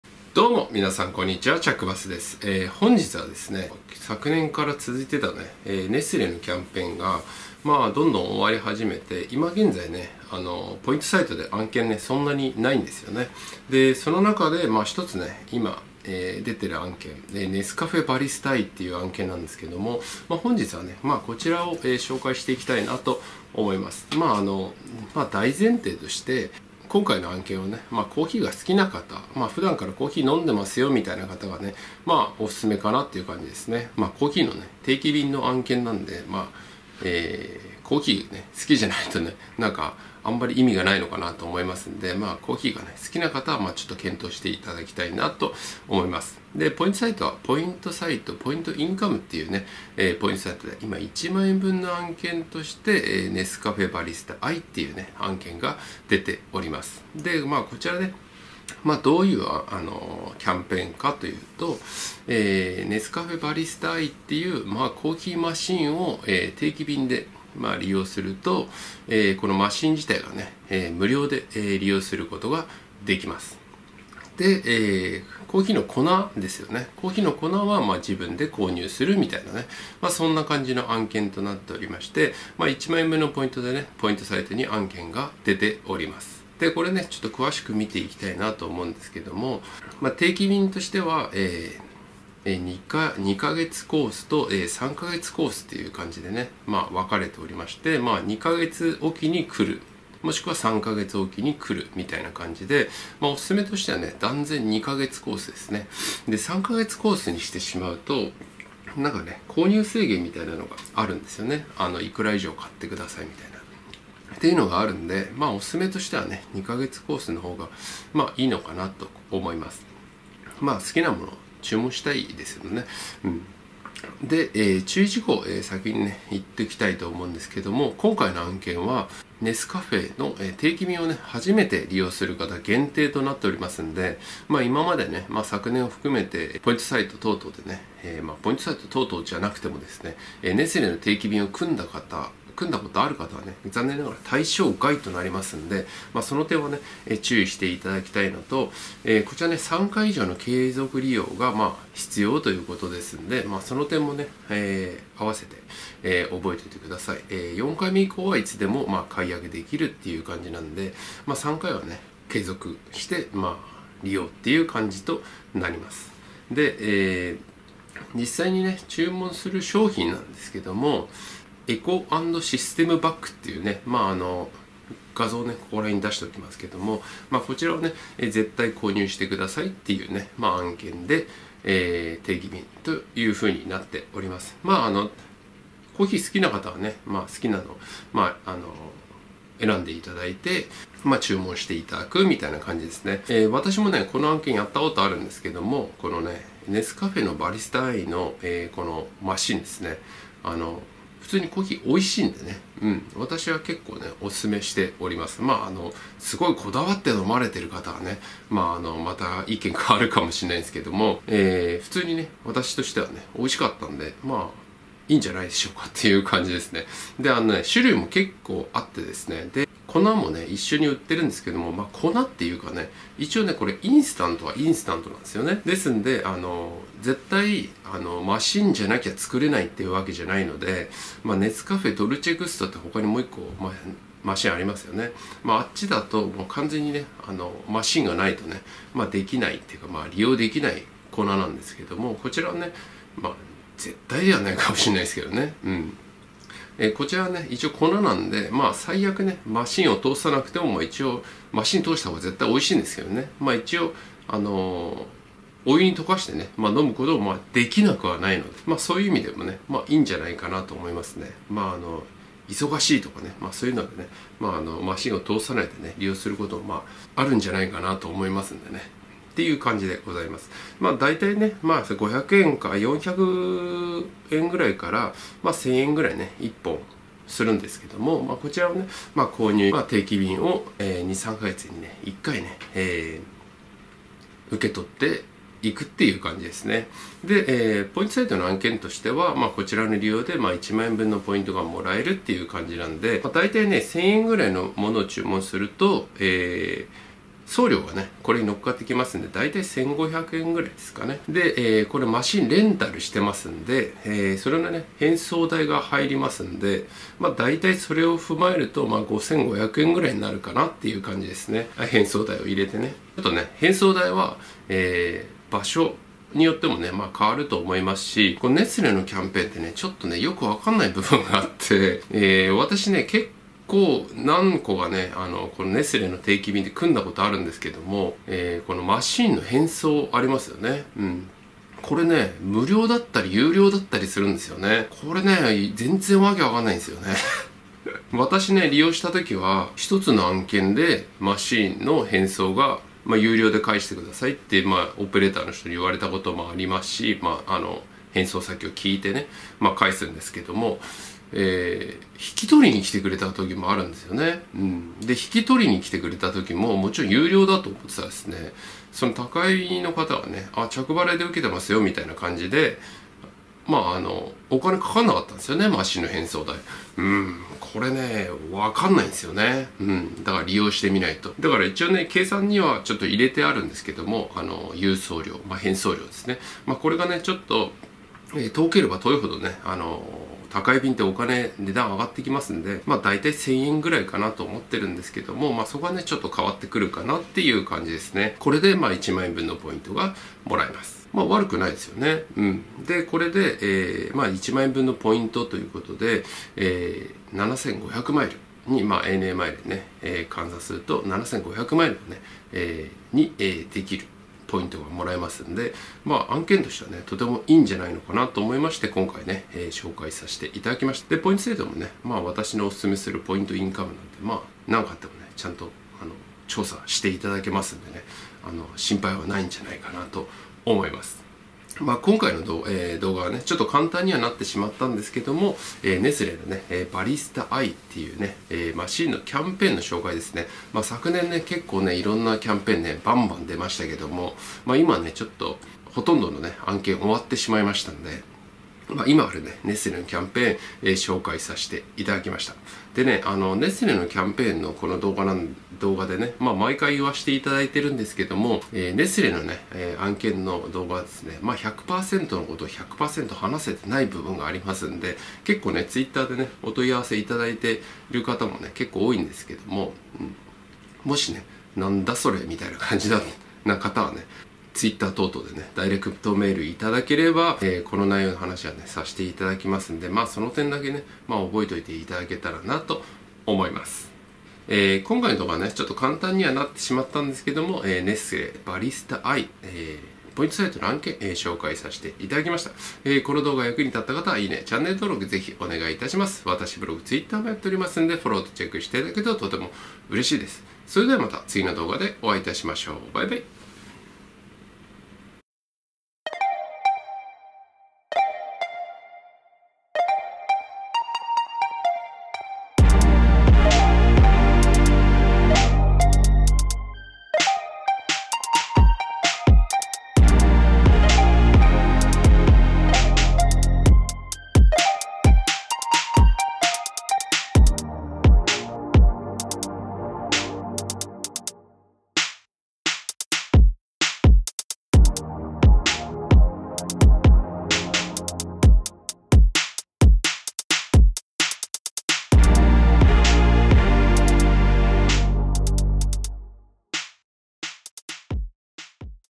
【ポイントサイト案件】残り少ないネスレ案件！バリスタ・アイ定期便利用案件解説！！
２０１９年たくさんあったネスレ案件ですが２０２０年に入りほとんど終了しました。残り少ないネスレ案件ですが個人的にはバリスタアイ案件はコーヒーも美味しくかなりオススメです！

ポイントサイト・ポイントインカムURL

・撮影機材

企業の方へ
私はニューヨークで働けるお仕事やツアーなどを募集しています。ありましたらぜひお声がけお願いします。